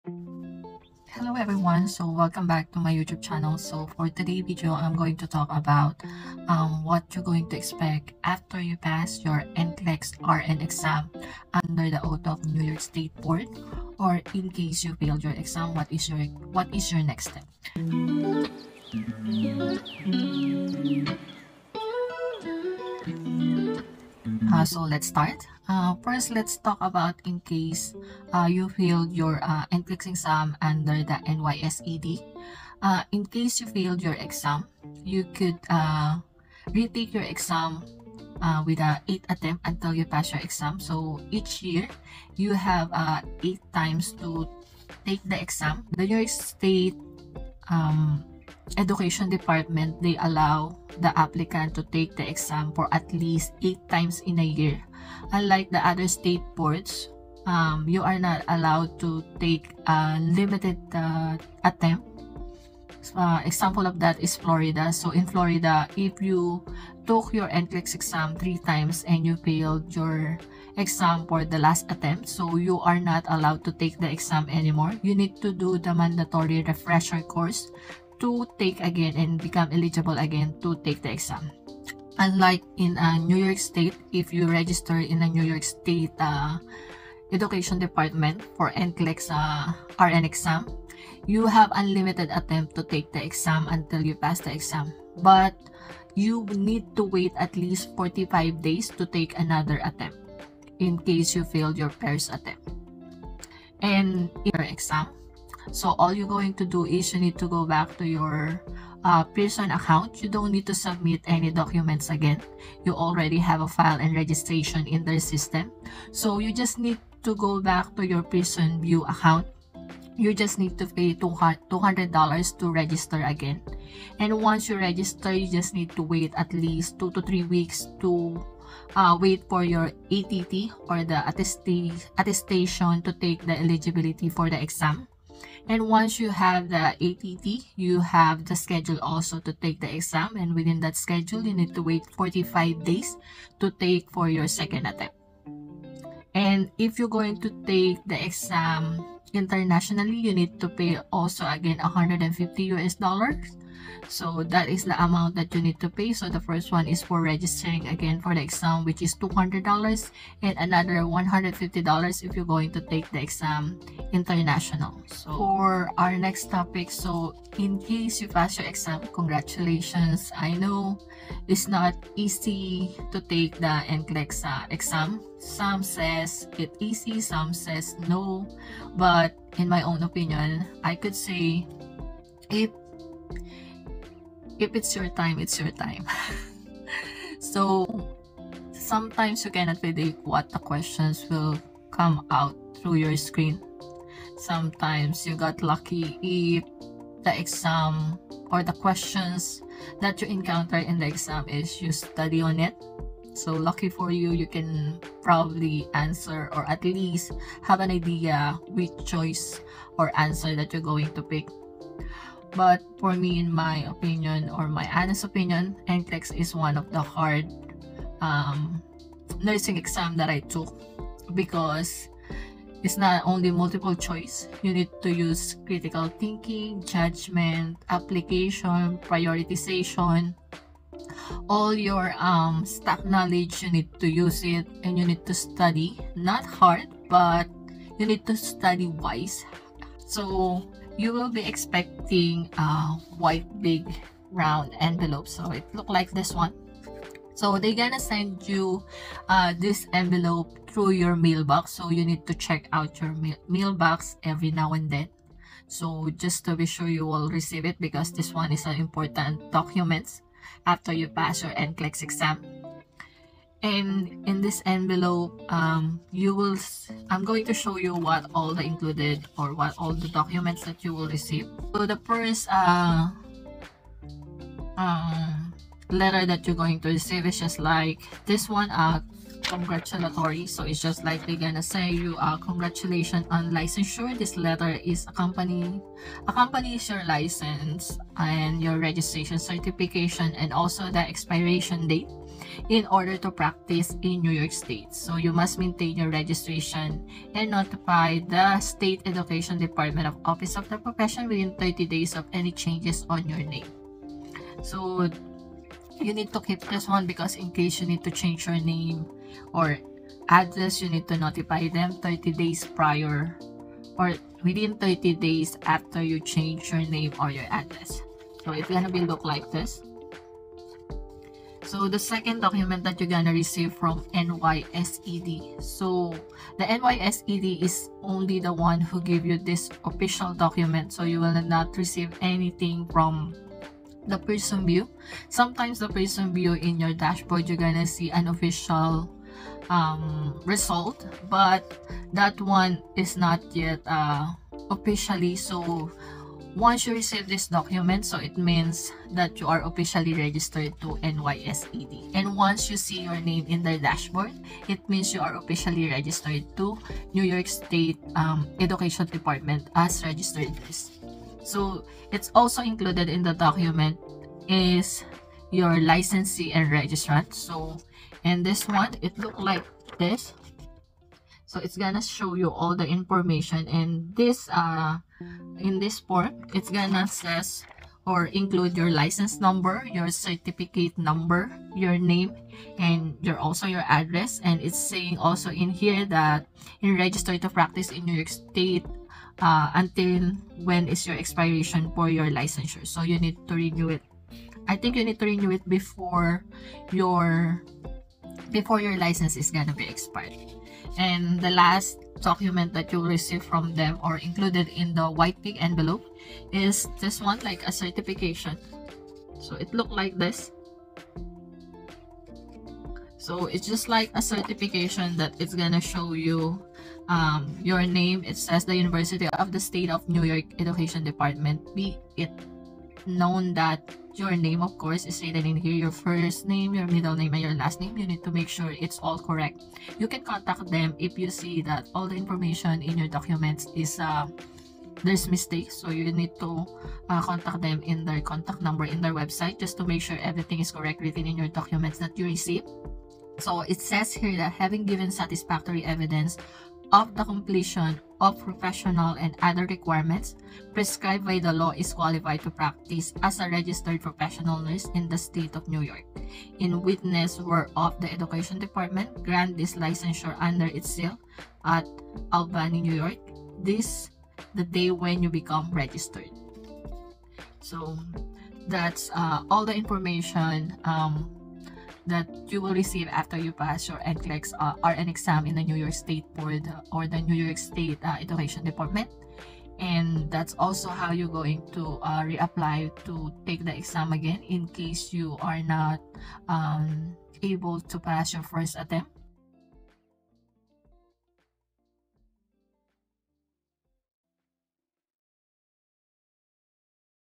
NEW YORK NCLEX-RN Pass/Fail | Rules & Step to Retaking| Next Step after Passing!
Steps How to Re-apply to take the NCLEX-RN | NYSED
1. Go to your Pearson Vue account
2. Click the Registration for the NCLEX-RN US
3. Pay the $200 USD
4. Wait 2-3 weeks for ATT thru your email.
5. Schedule the test date after 45 days from the date you took your NCLEX-RN exam.

Next Step after Passing the NCLEX- RN
1. After passing the NCLEX-RN, you will see unofficial results from your Pearson Vue Account.
2. Within 2-3 weeks your name will have appeared in the NYSED dashboard that will show your Name, License#, and Date of Licensure. Where you can request a license verification
3. You will be going to receive a white big envelope with a Congratulation Letter, License Certification within 10 - 30 business days from the date that takes your NCLEX-RN exam.